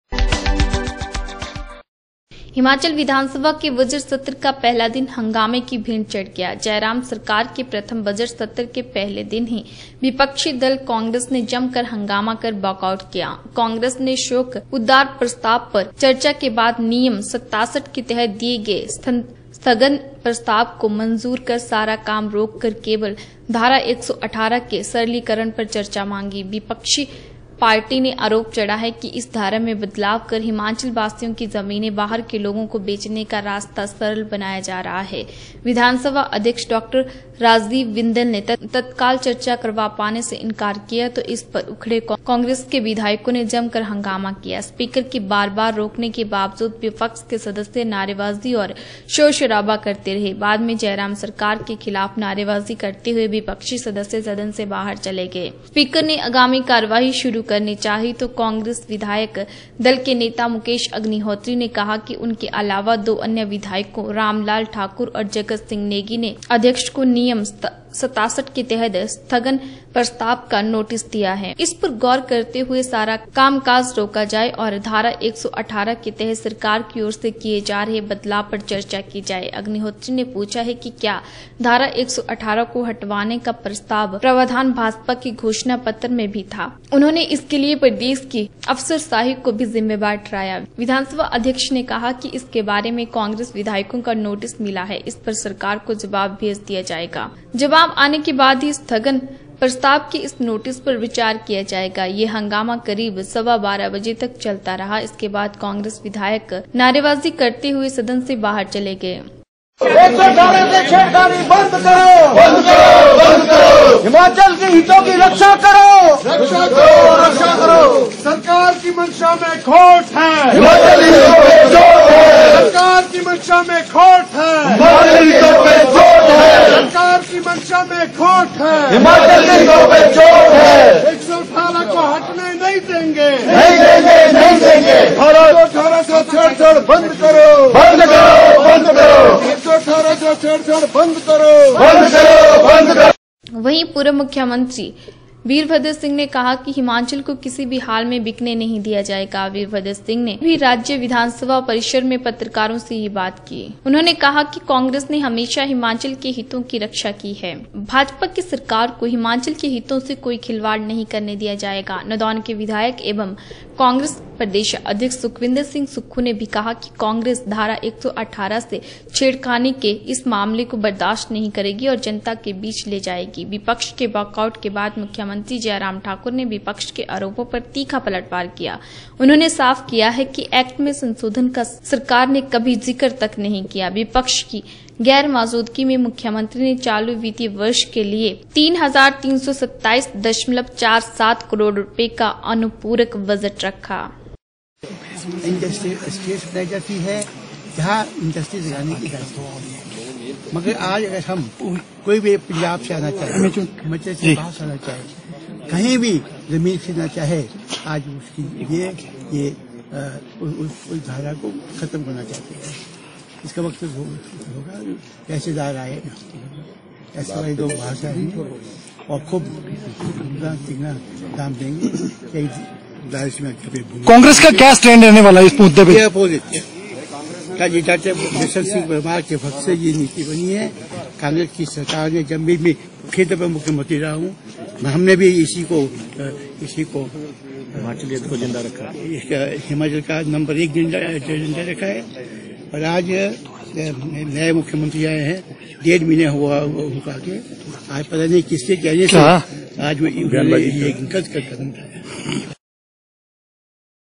बजट सत्र का पहला ही दिन हंगामे की भेंट , विपक्षी दल कांग्रेस ने जमकर हंगामा कर किया वाकआउट
हिमाचल विधानसभा के बजट सत्र का पहला ही दिन हंगामे की भेंट चढ़ गया। जयराम सरकार के प्रथम बजट सत्र के पहले दिन ही विपक्षी दल कांग्रेस ने जमकर हंगामा कर वाकआउट किया।कांग्रेस ने शोक उद्गार प्रस्ताव पर चर्चा के बाद नियम 67 के तहत दिए स्थगन प्रस्ताव को मंजूर कर सारा काम रोककर केवल धारा 118 के सरलीकरण पर चर्चा मांगी।